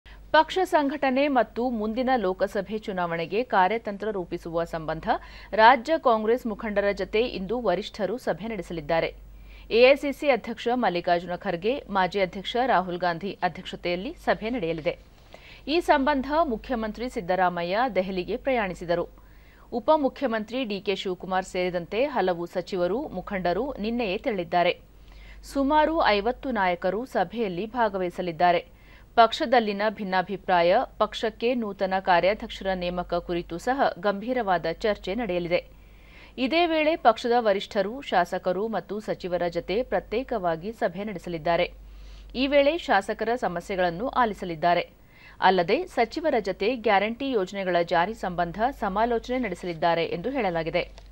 ಮುಖ್ಯಮಂತ್ರಿ ಸಿದ್ದರಾಮಯ್ಯ ದೆಹಲಿಗೆ ಪ್ರಯಾಣ; ಕಾಂಗ್ರೆಸ್‌ ಮುಖಂಡರ ಜತೆ ಇಂದು ವರಿಷ್ಠರ ಸಭೆ
ಪಕ್ಷ ಸಂಘಟನೆ ಮತ್ತು ಮುಂದಿನ ಲೋಕಸಭೆ ಚುನಾವಣೆಗೆ ಕಾರ್ಯತಂತ್ರ ರೂಪಿಸುವ ಸಂಬಂಧ ರಾಜ್ಯ ಕಾಂಗ್ರೆಸ್‌ ಮುಖಂಡರ ಜತೆ ಇಂದು ವರಿಷ್ಠರು ಸಭೆ ನಡೆಸಲಿದ್ದಾರೆ.
ಎಐಸಿಸಿ ಅಧ್ಯಕ್ಷ ಮಲ್ಲಿಕಾರ್ಜುನ ಖರ್ಗೆ, ಮಾಜಿ ಅಧ್ಯಕ್ಷ ರಾಹುಲ್ ಗಾಂಧಿ ಅಧ್ಯಕ್ಷತೆಯಲ್ಲಿ ಸಭೆ ನಡೆಯಲಿದೆ.
ಈ ಸಂಬಂಧ ಮುಖ್ಯಮಂತ್ರಿ ಸಿದ್ದರಾಮಯ್ಯ ದೆಹಲಿಗೆ ಪ್ರಯಾಣಿಸಿದರು. ಉಪಮುಖ್ಯಮಂತ್ರಿ ಡಿ.ಕೆ.ಶಿವಕುಮಾರ್‌ ಸೇರಿದಂತೆ ಹಲವು ಸಚಿವರು, ಮುಖಂಡರು ನಿನ್ನೆಯೇ ತೆರಳಿದ್ದಾರೆ.
ಸುಮಾರು 50 ನಾಯಕರು ಸಭೆಯಲ್ಲಿ ಭಾಗವಹಿಸಲಿದ್ದಾರೆ. ಪಕ್ಷದಲ್ಲಿನ ಭಿನ್ನಾಭಿಪ್ರಾಯ, ಪಕ್ಷಕ್ಕೆ ನೂತನ ಕಾರ್ಯಾಧ್ಯಕ್ಷರ ನೇಮಕ ಕುರಿತು ಸಹ ಗಂಭೀರವಾದ ಚರ್ಚೆ ನಡೆಯಲಿದೆ.
ಇದೇ ವೇಳೆ ಪಕ್ಷದ ವರಿಷ್ಠರು, ಶಾಸಕರು ಮತ್ತು ಸಚಿವರ ಜತೆ ಪ್ರತ್ಯೇಕವಾಗಿ ಸಭೆ ನಡೆಸಲಿದ್ದಾರೆ.
ಈ ವೇಳೆ ಶಾಸಕರ ಸಮಸ್ಯೆಗಳನ್ನು ಆಲಿಸಲಿದ್ದಾರೆ. ಅಲ್ಲದೇ, ಸಚಿವರ ಜತೆ ಗ್ಯಾರಂಟಿ ಯೋಜನೆಗಳ ಜಾರಿ ಸಂಬಂಧ ಸಮಾಲೋಚನೆ ನಡೆಸಲಿದ್ದಾರೆ ಎಂದು ಹೇಳಲಾಗಿದೆ.